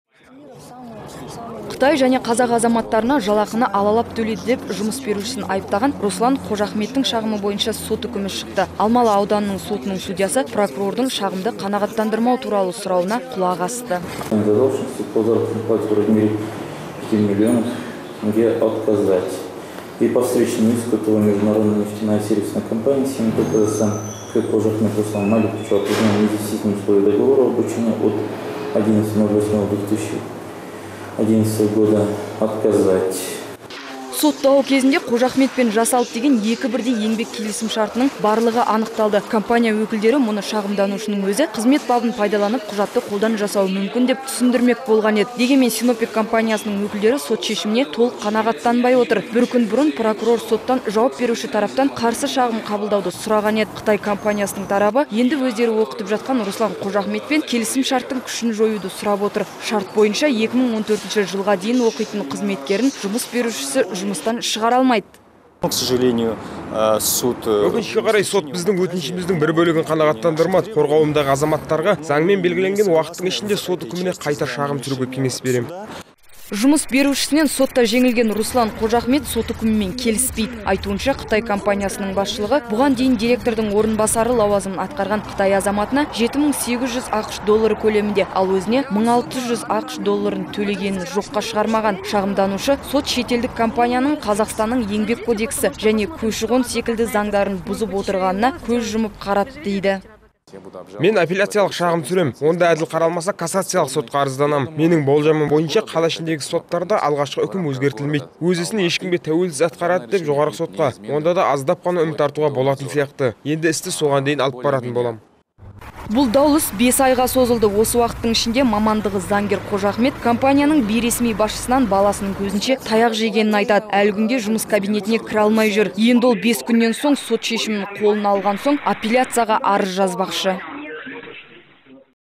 Сот Қытайлық жұмыс берушіні ақтап алды / 1612
Қытай және қазақ азаматтарына жалақыны алалап төлейді деп жұмыс берушісін айыптаған Руслан Қожахметтің шағымы бойынша сот үкімі шықты. Алмалы аудандық сотының судьясы прокурордың шағымды қанағаттандырмау туралы сұрауына құлақ асты.